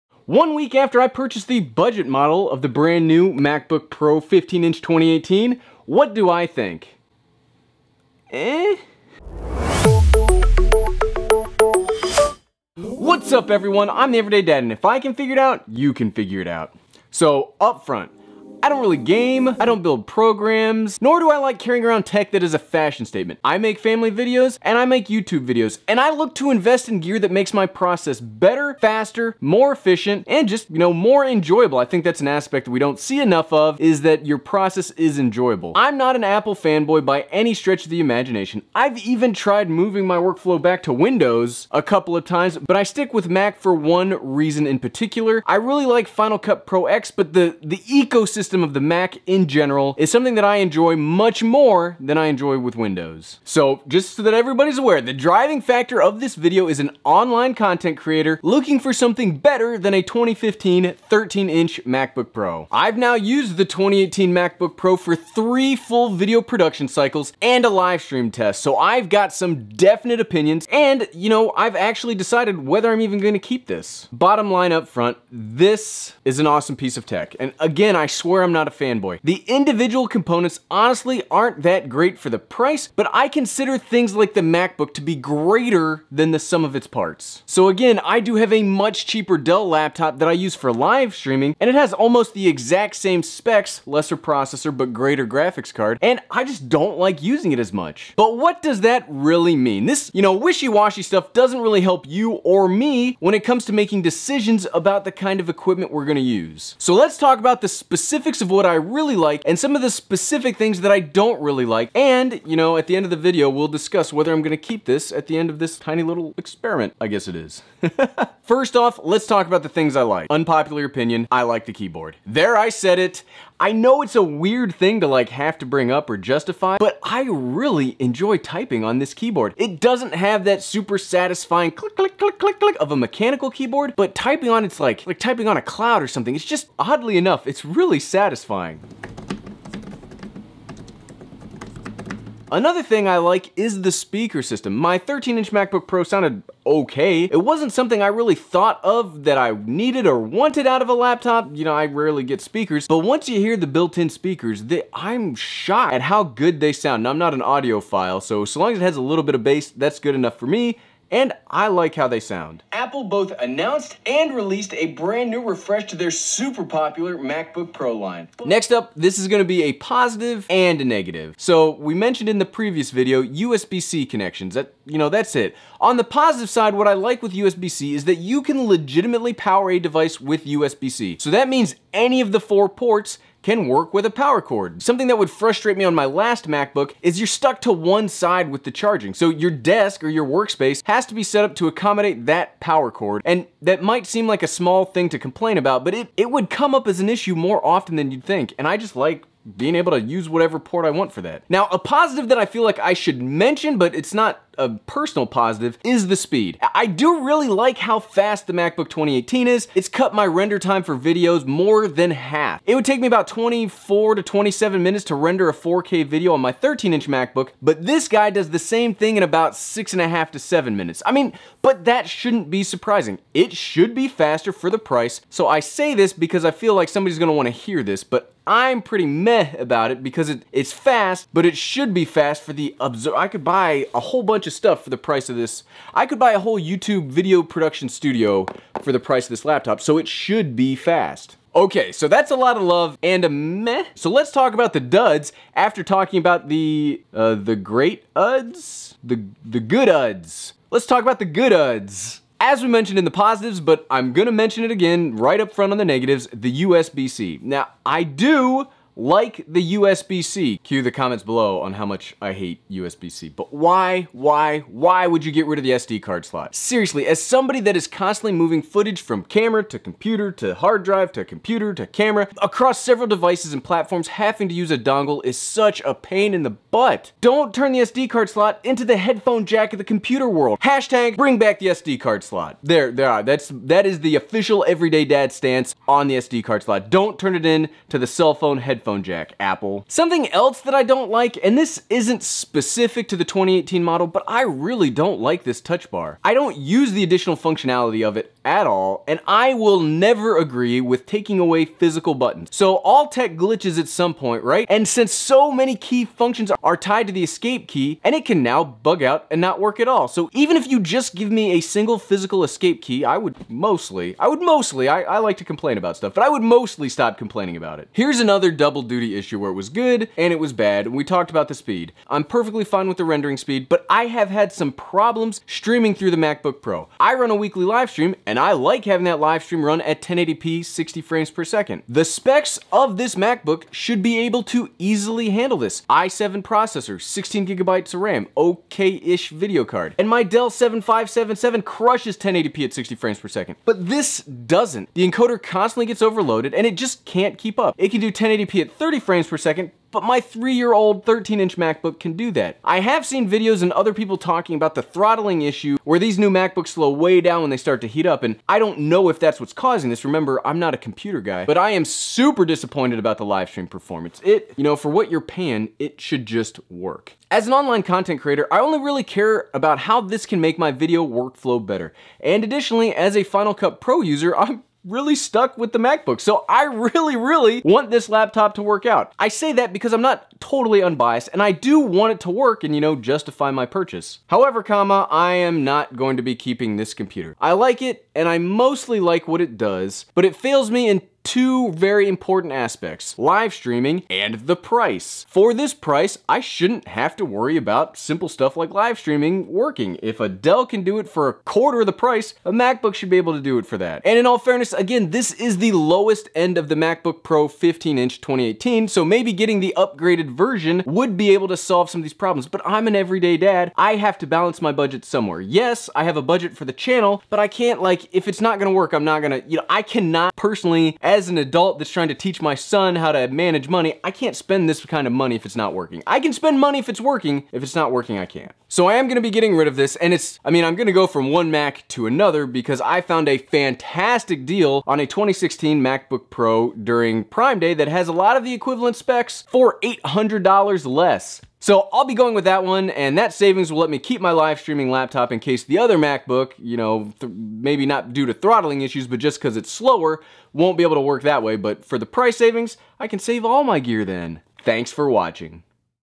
The "BUDGET" MacBook Pro 15 2018 ONE WEEK (ISH) LATER! Worth Keeping?!
Today we're doing our follow up to Mondays video about the brand new MacBook Pro 15 inch!  I really like working with MacBooks and I'm pretty excited to have used this one for about a week.

Is it worth keeping?  And is it worth the premium?